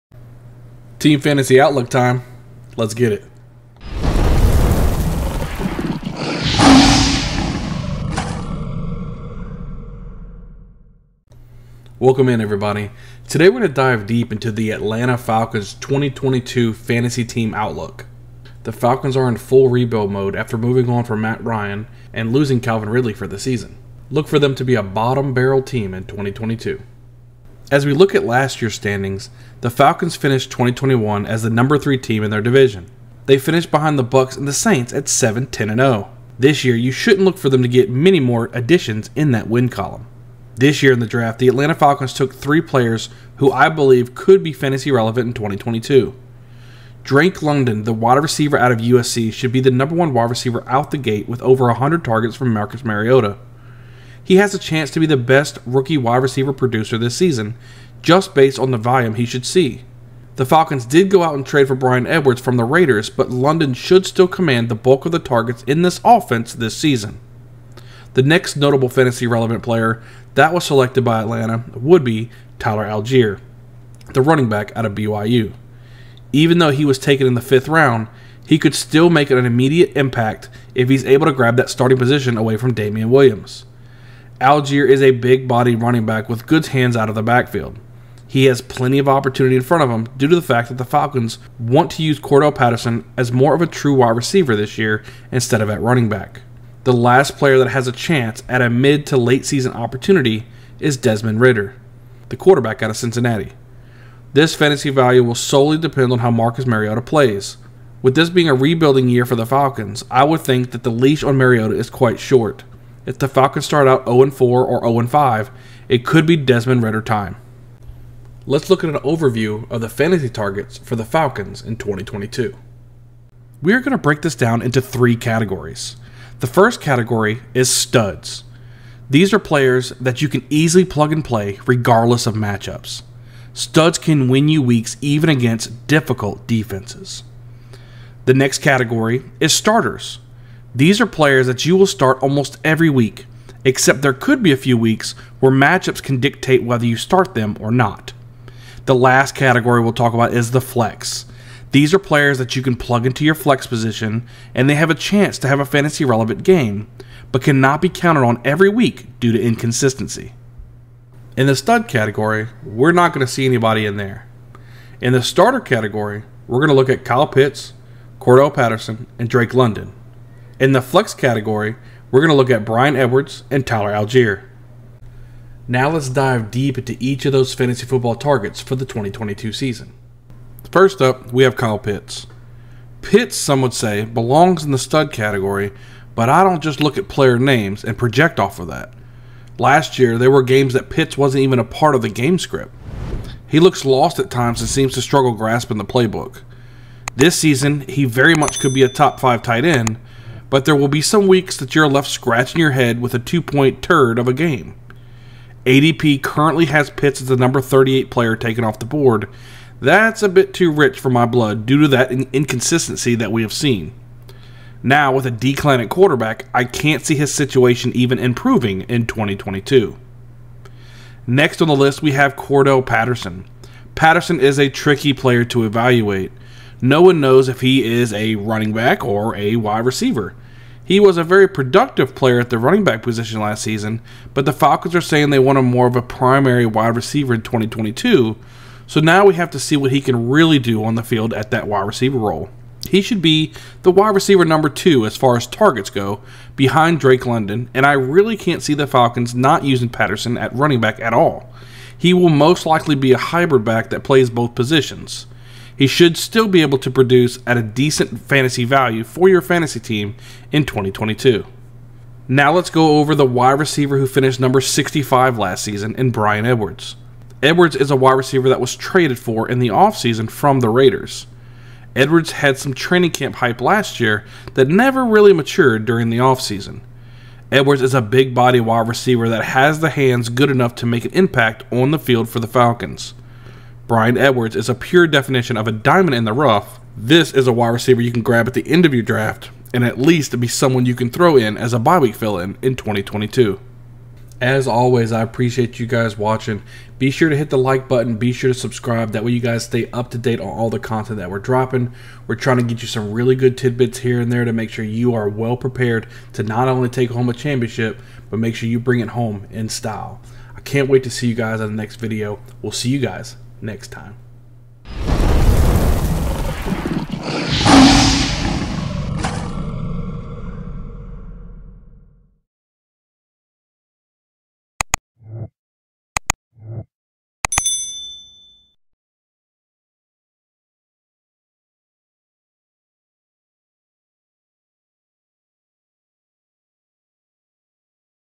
Atlanta Falcons Team Fantasy Outlook
Breaking down the fantasy football relevant players of the 2022 Atlanta Falcons.

Delivering all the fantasy football information for you to be prepared for your fantasy draft and the entire season! Time to win some fantasy football championships!

SOCIALS!